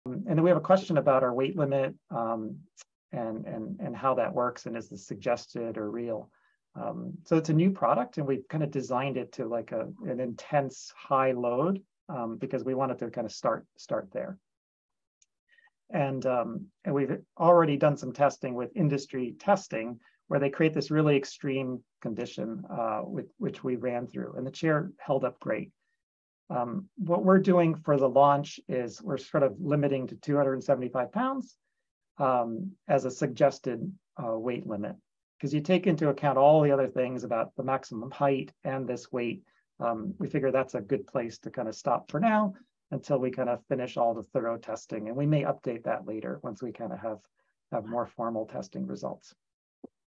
Movably Pro's Weight Capacity
Mark explains why we chose to list the max weight as 275lbs and that this number may be updated after more testing. Clip from our 10.17.23 livestream.